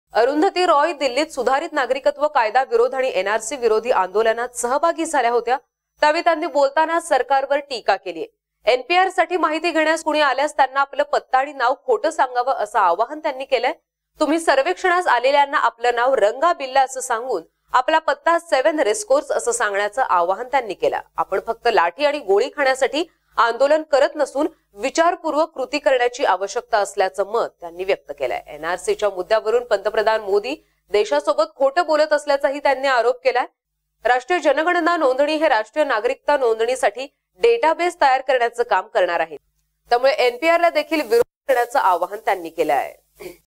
ज्येष्ठ लेखिका अरुंधती रॉय यांचे आवाहन | MKN MARATHI
अरुंधती रॉय यांचे  केंद्र सरकारवर टीकास्त्र
एनपीआरसाठी माहिती घेण्यास आलेल्याला तुम्ही खोटी माहिती पुरवा
ज्येष्ठ लेखिका अरुंधती रॉय यांचे आवाहन

सुधारीत नागरीकत्व कायदा आणि एनआरसी विरोधात सुरू असलेल्या आंदोलनात लेखिका आणि सामाजिक कार्यकर्त्या  अरुंधती रॉय यांनीही आता  उडी घेतलीयं  याच विरोधात बोलतांना त्यांनी एका वादग्रस्त वक्त्यव केलंय . एनपीआर सर्वेक्षणाच्या वेळी आपले नाव "रंगा बिल्ला सांगा" असे आवाहन त्यांनी उपस्थितांना केले.